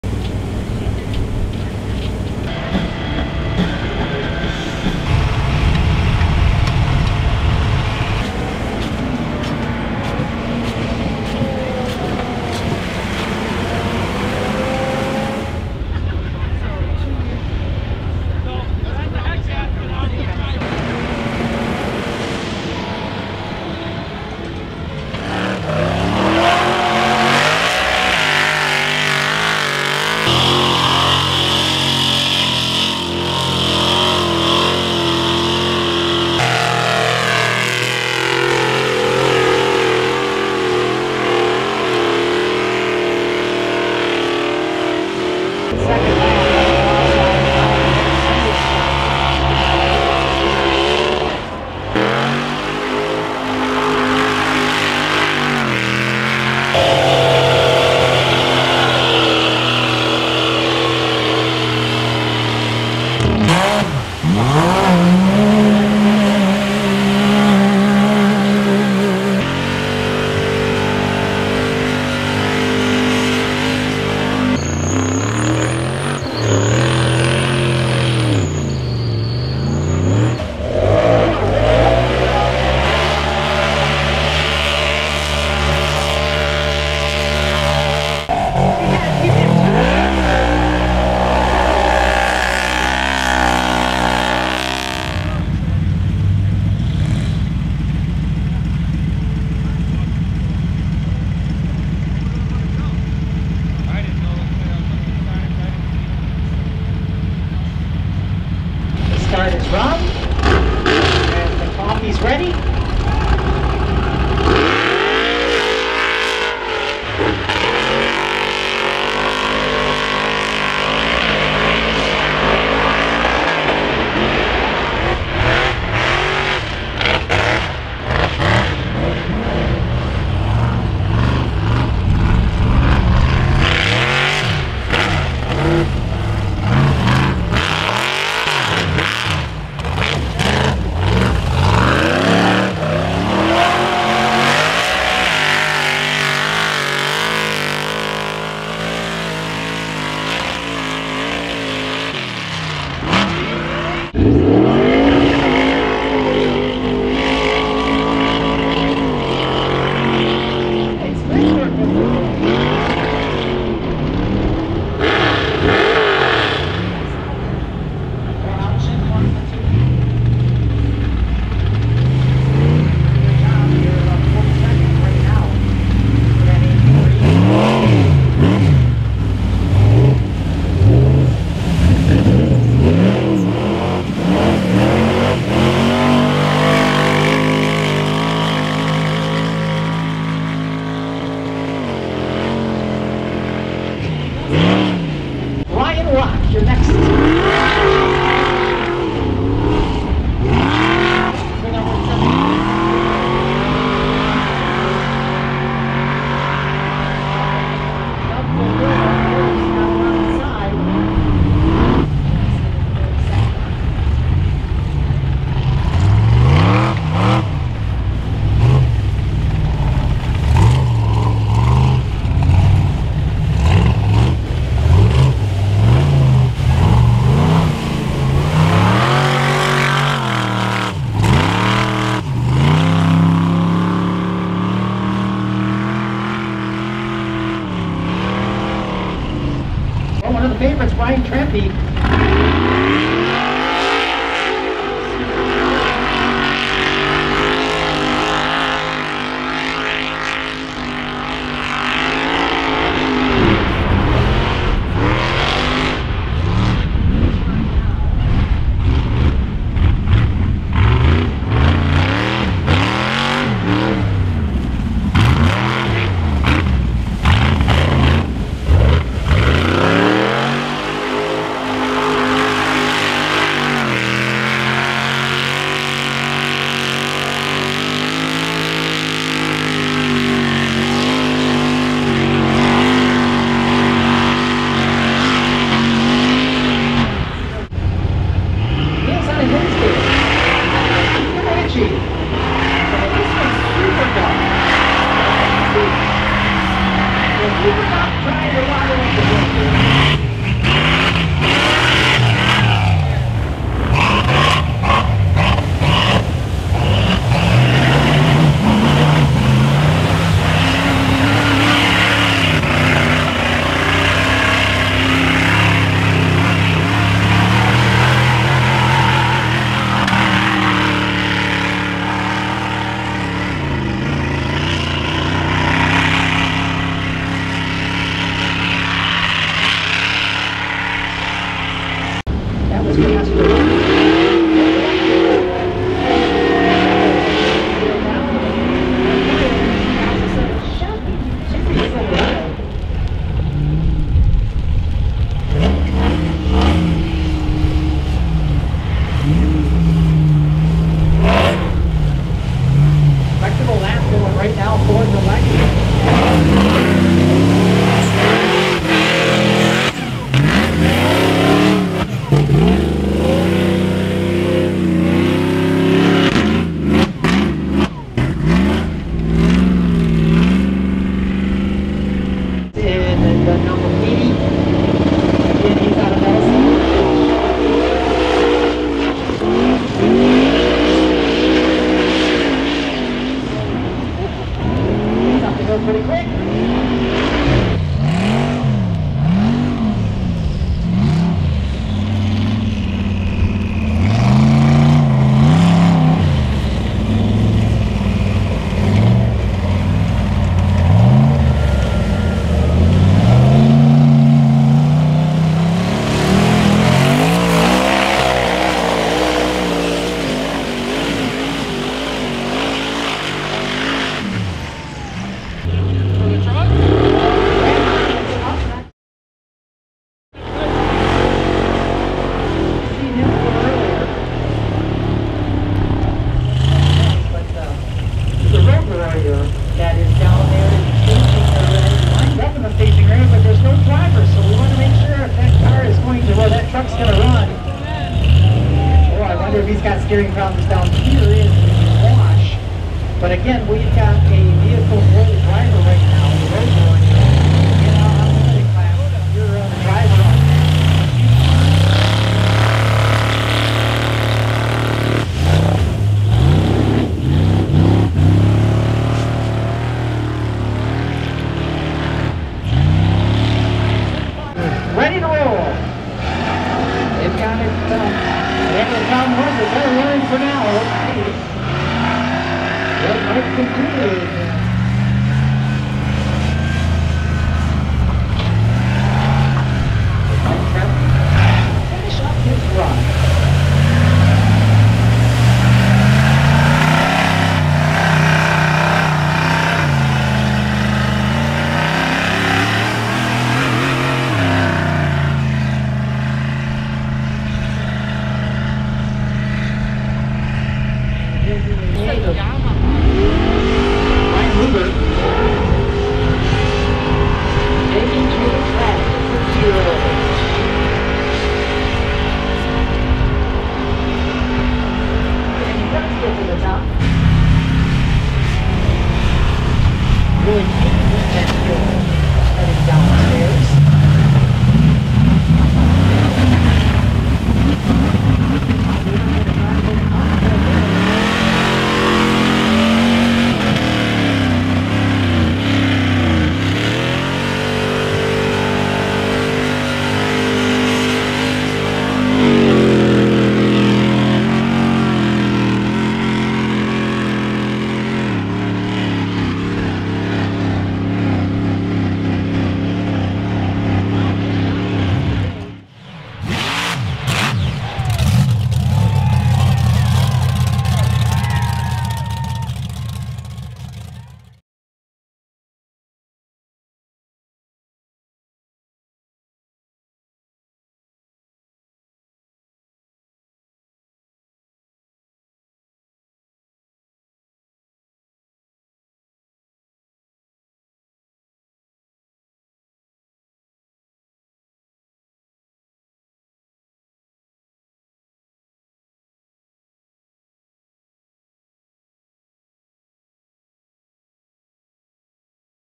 Unlimited Class- Snow Snake Hill Climb 22
Watch Unlimited class take on the snow snake hill climb. Rain made things interesting and flooded spots on the course. Way up top was a few turns I couldn't see but the real action starts on the last turn into the final hill climb. Stock class, modified class and SXS class coming soon.